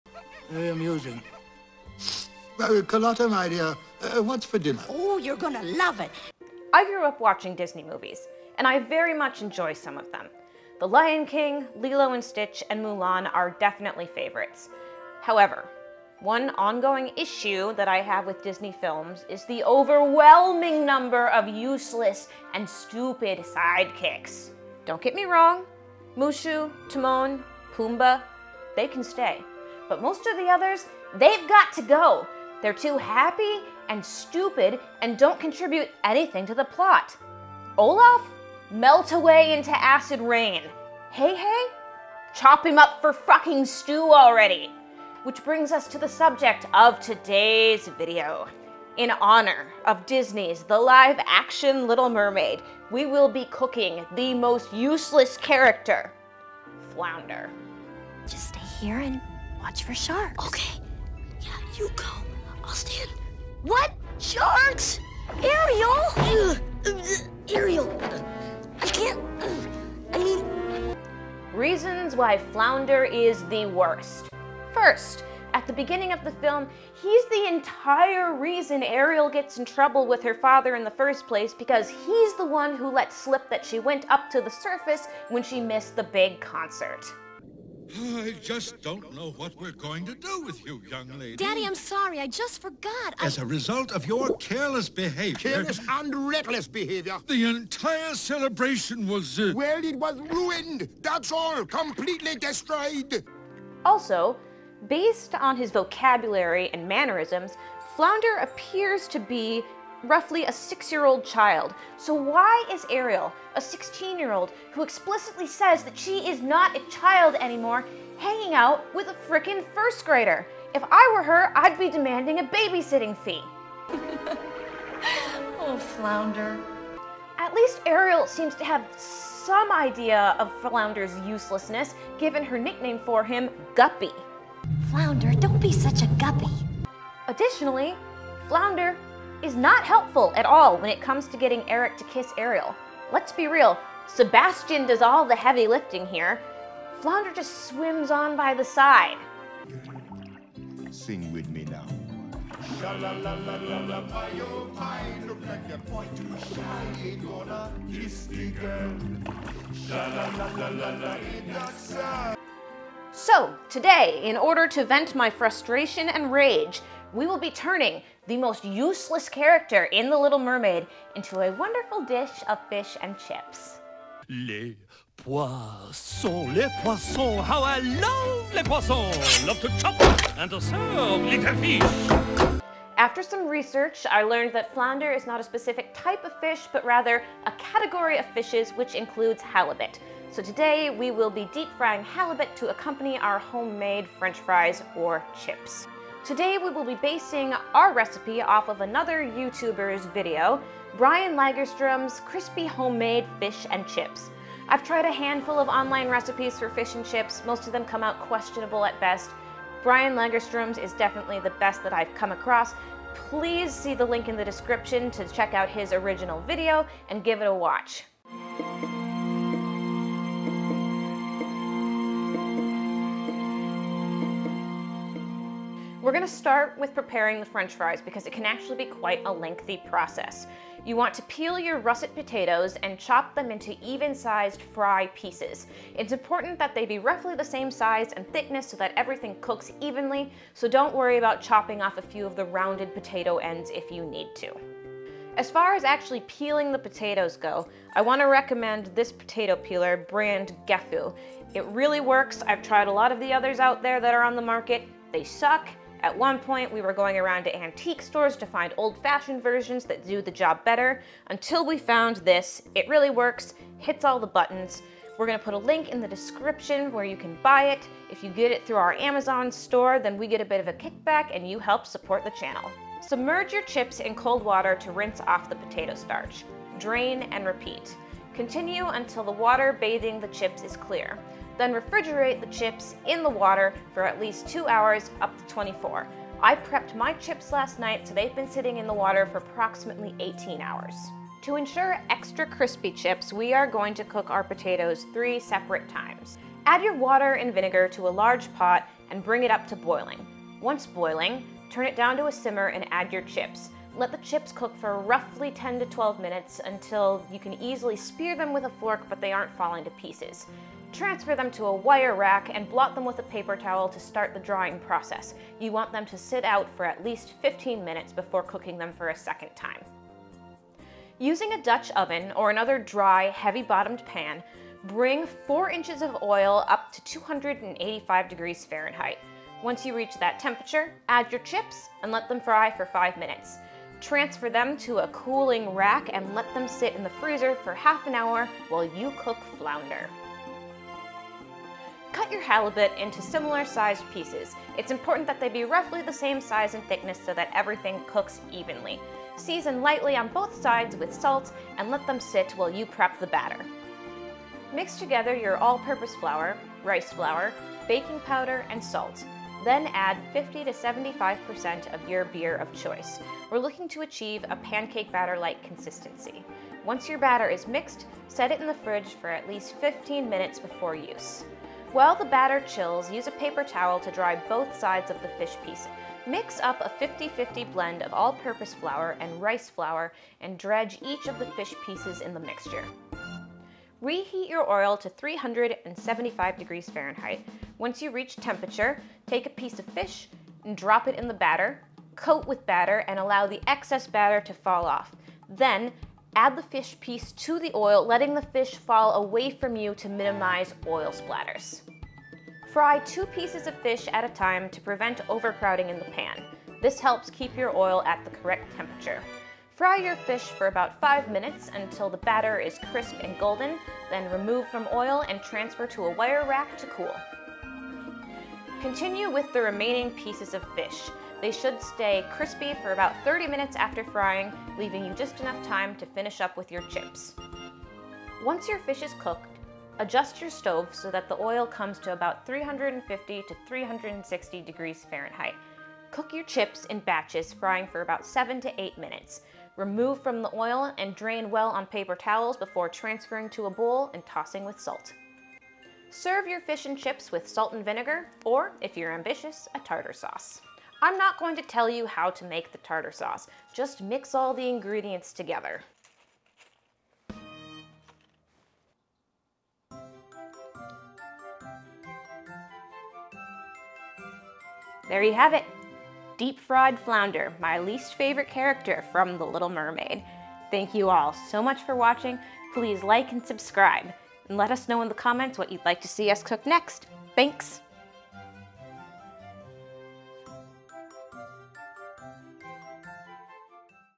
How to Cook Fish & Chips - The Little Mermaid
Today we get revenge on one of our least favorite characters... and share a good fish and chips recipe!

Our Amazon Store:
Gefu Peeler